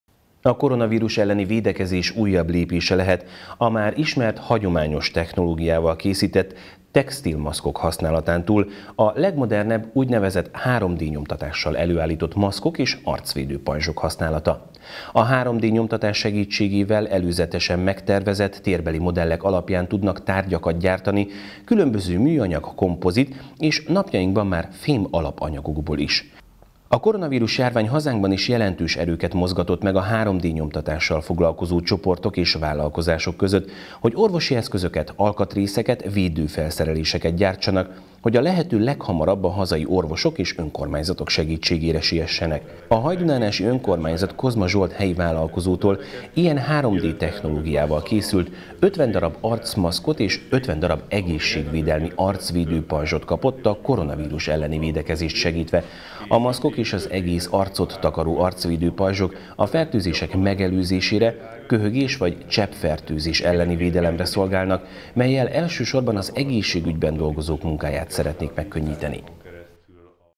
Hajdúnánási Televízió: 3D-s nyomtatással készült maszkokat ajánlott fel egy vállalkozó 2020.06.05.
A koronavírus elleni védekezés újabb lépése lehet a már ismert, hagyományos technológiával készített textil maszkok használatán túl, a legmodernebb, úgynevezett 3D nyomtatással előállított maszkok és arcvédő pajzsok használata. A 3D nyomtatás segítségével előzetesen megtervezett térbeli modellek alapján tudnak tárgyakat gyártani különböző műanyag, kompozit, és napjainkban már fém alapanyagból is. A koronavírus járvány hazánkban is jelentős erőket mozgatott meg a 3D-nyomtatással foglalkozó csoportok és vállalkozások között, hogy orvosi eszközöket, alkatrészeket, védő felszereléseket gyártsanak, hogy a lehető leghamarabb a hazai orvosok és önkormányzatok segítségére siessenek.A Hajdúnánási Önkormányzat Kozma Zsolt helyi vállalkozótól ilyen 3D technológiával készült 50 darab arcmaszkot és 50 darab egészségvédelmi arcvédő pajzsot kapott a koronavírus elleni védekezést segítve. A maszkok és az egész arcot takaró arcvédő pajzsok a fertőzések megelőzésére, köhögés vagy cseppfertőzés elleni védelemre szolgálnak, mellyel elsősorban az egészségügyben dolgozók munkáját szeretnék megkönnyíteni.